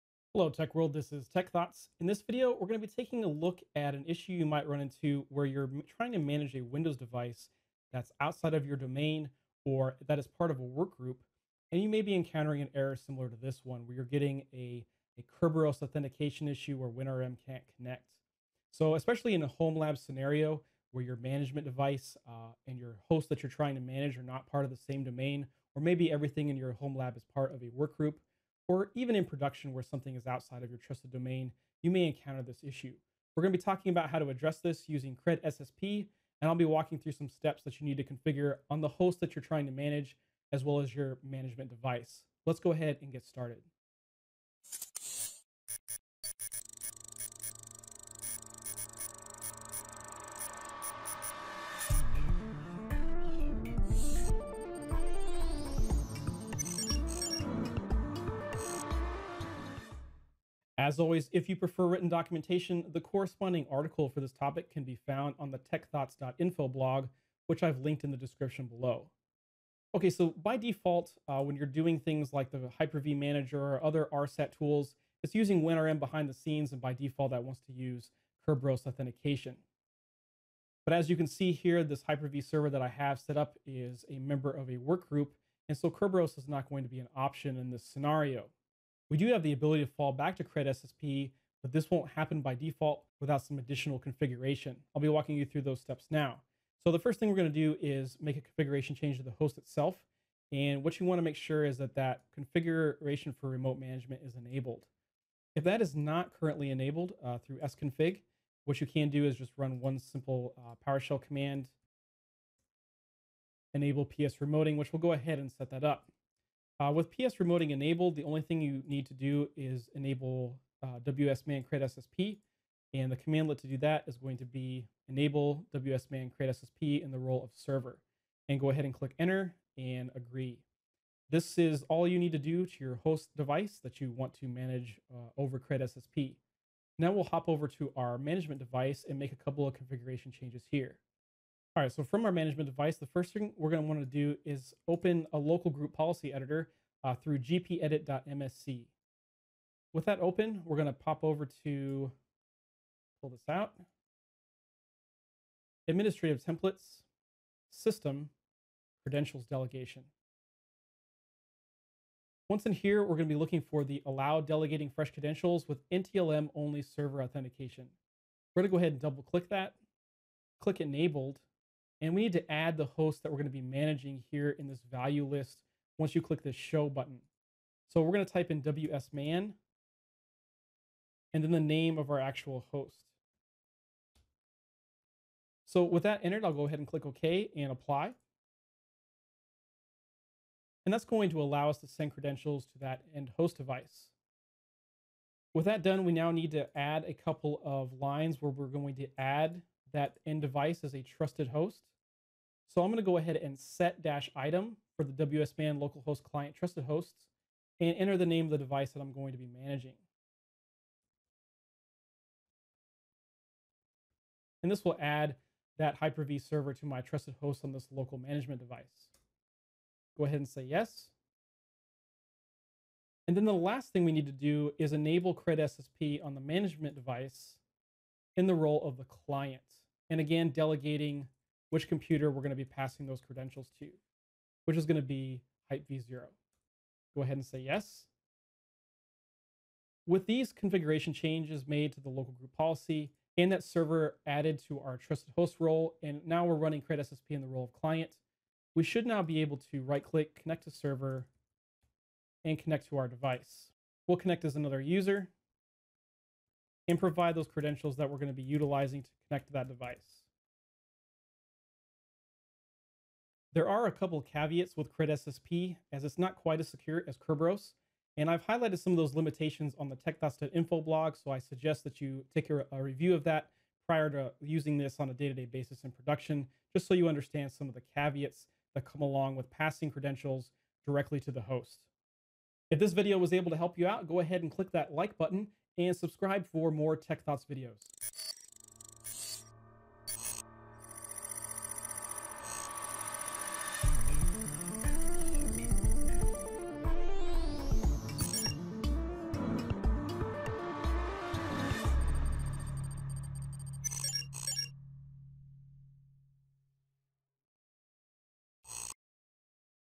Managing windows devices and Hyper-V hosts with CredSSP
Manage hosts outside your domain or workgroup devices using CredSSP. How to guide for enabling CredSSP on both host and management device. Connect to and manage Hyper-V with CredSSP. Resolve Kerberos authentication issues.